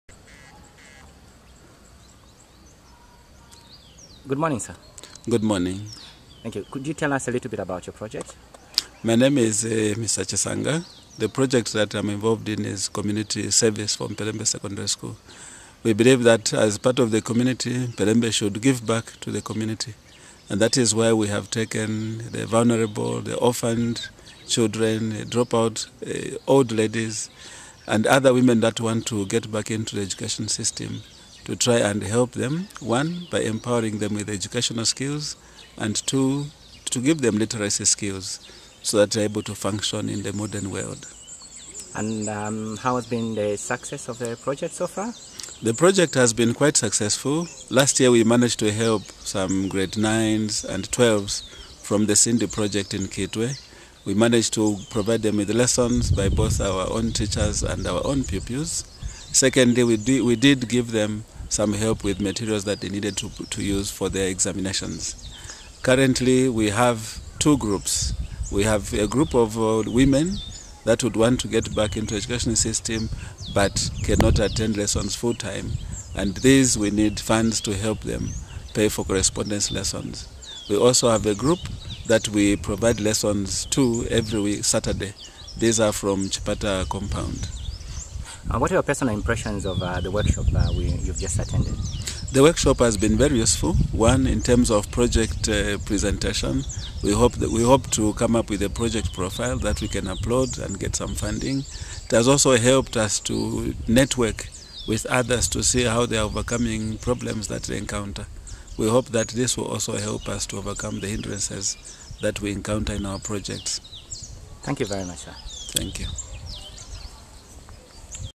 Mr Chisenga of Mpelembe Secondary School
Here is Yese Bwalya of Careermate interviewing  Mr Chisenga of Mpelembe Sec. School together practicing how to make short video interviews and use them to report on their projects and use video for project marketing.  They are practicing during the Akvo workshop for the Connect4Change program partners. 22 February 2012, Kitwe, Zambia.